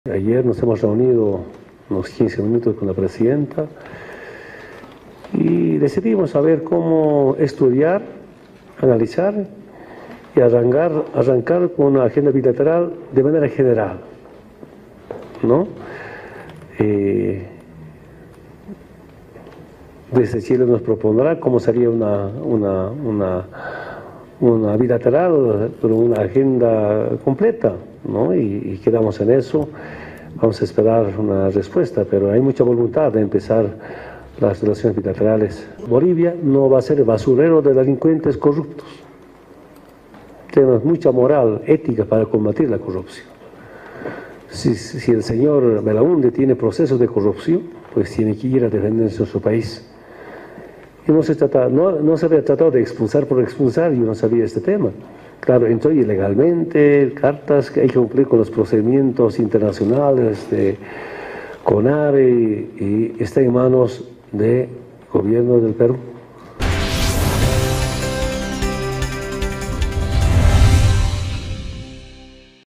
Morales dijo esperar de Chile una propuesta sobre la agenda bilateral "completa"
El presidente de Bolivia, Evo Morales, afirmó hoy que su país espera de Chile una propuesta para trabajar en una agenda bilateral "completa", que incluye el tema del reclamo marítimo, como acordó con su par chilena, Michelle Bachelet, en el cierre de ayer de la cumbre de la Celac en Costa Rica.
"Decidimos cómo estudiar, analizar y arrancar con una agenda bilateral de manera general. Desde Chile nos propondrán cómo sería una bilateral con una agenda completa y quedamos en eso", declaró Morales en rueda de prensa, al comentar el encuentro con su homóloga chilena en la cumbre de la Comunidad de Estados Latinoamericanos y Caribeños (Celac).
El mandatario reiteró que "vamos a esperar una respuesta, pero hay mucha voluntad de empezar las relaciones bilaterales".
Según Morales, Bachelet le dijo que, al margen de tener un tema pendiente (en referencia a la demanda marítima boliviana), Bolivia y Chile son naciones vecinas que no pueden seguir enfrentadas, sino que deben avanzar "de manera conjunta en todos los temas".
Ayer, el canciller de Bolivia, David Choquehuanca, dijo que los dos mandatarios hablaron de la posibilidad de retomar una agenda de 13 puntos, "sin exclusiones".
Uno de los puntos de esa agenda se refiere a la reivindicación boliviana de recuperar un acceso soberano al Pacífico, tras perder su litoral marítimo a manos de tropas chilenas tras la invasión de su territorio y una guerra que se extendió entre 1879 y 1883.
Choquehuanca contradijo así a su homólogo de Chile, Heraldo Muñoz, que poco antes había asegurado que en la reposición de esa agenda quedaba excluido el apartado referido al tema marítimo.
Las relaciones entre Bolivia y Chile se tensaron cuando en abril de 2013 el gobierno de Morales demandó al país vecino ante la Corte Internacional de Justicia (CIJ) de La Haya por la reivindicación marítima.
Bolivia pide a la CIJ un fallo que obligue a Chile a negociar en firme y de buena fe su centenario reclamo, con el argumento de que ha tenido propuestas de gobiernos chilenos reconociendo la controversia y planteando soluciones, pero nunca se concretaron los acuerdos.
Bolivia perdió en la Guerra del Pacífico 400 kilómetros de costa y 120.000 kilómetros cuadrados de su territorio ante tropas chilenas y desde entonces reclama la restitución de ese acceso.
Chile, que considera que todos los asuntos limítrofes con Bolivia están resueltos desde 1904, cuando ambos países suscribieron un tratado de paz y amistad, objetó la competencia de La Haya para pronunciarse sobre el asunto, por tratarse de un litigio resuelto a través de un tratado antes de la creación del tribunal de La Haya, en 1948.
Mientras se espera que la Corte se pronuncie a mediados de año sobre esa objeción, ambos países han desarrollado campañas internacionales para difundir sus argumentos.